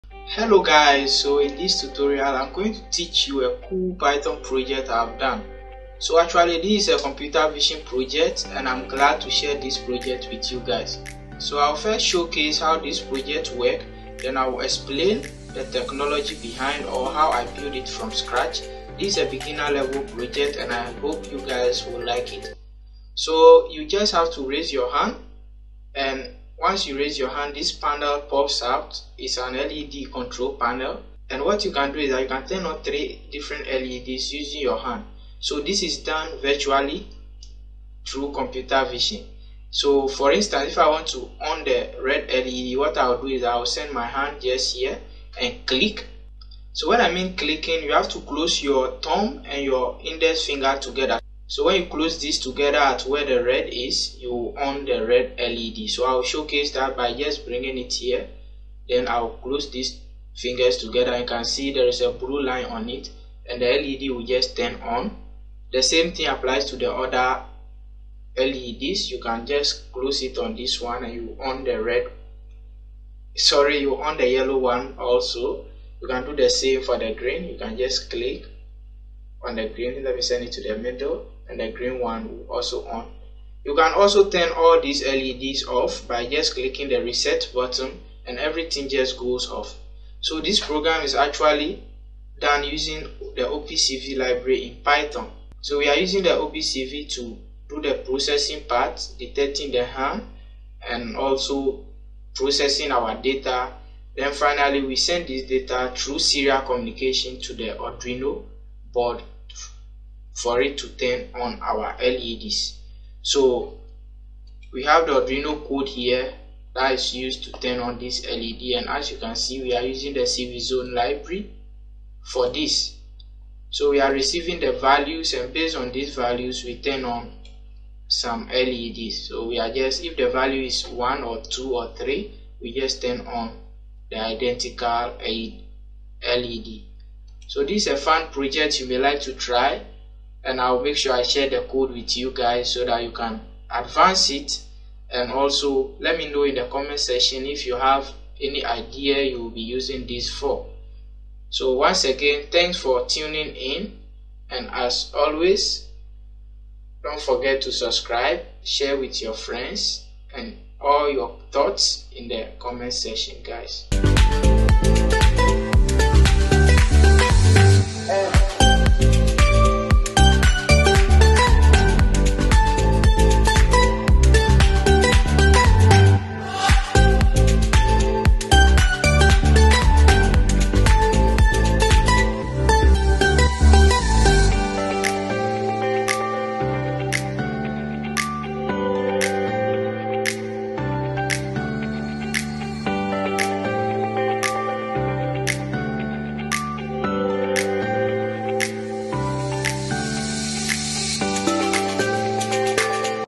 Control Traffic Lights Using Hand Detection | OpenCV Project | Python Hub
In this video, we will be demonstrating how to control traffic lights using hand detection with the OpenCV library. We will be using a webcam to capture video footage of a person's hand and using that information to control the traffic lights. This project is a great example of how computer vision and machine learning can be used in real-world applications.